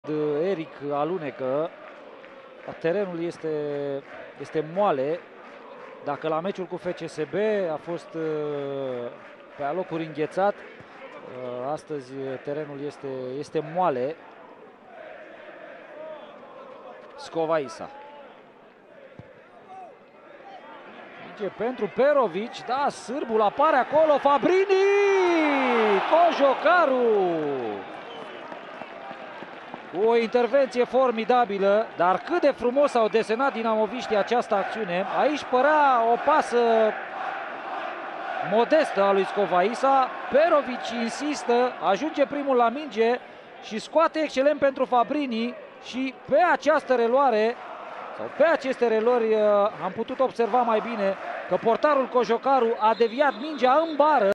FC Voluntari - Dinamo: Cojocaru deviaza in bara sutul lui Fabrini
FC Voluntari - Dinamo: Cojocaru deviaza in bara sutul lui Fabrini I Etapa 24,  Liga 1, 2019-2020

FC Voluntari: Cojocaru - Struna, Pașcanu, Achim, Kocic - Gorobsov, Pablo de Lucas - Căpățînă, Eric de Oliveira, Ion Gheorghe - Simonovski
Rezerve: Alexandru Vlad, Rîmniceanu, Claudiu Belu, Bortoneanu, Moussa Sanoh, Țîră, Tudorie
Antrenor: Mihai Teja

Dinamo: Piscitelli - Kostrna, Mihai Popescu, Ricardo Grigore, Skovajsa - Mrzljak, Răuță - Mihaiu, Fabbrini, Sorescu - Perovic
Rezerve: Straton, Puljic, Kilyen, Filip, Lazăr, Moldoveanu, Daniel Popa
Antenor: Dusan Uhrin

Stadion: „Anghel Iordănescu”
Arbitru: Adrian Cojocaru (Galați) / Asistenți: Ciprian Danșa (Satu Mare) și Marius Nicoară (Mehedinți)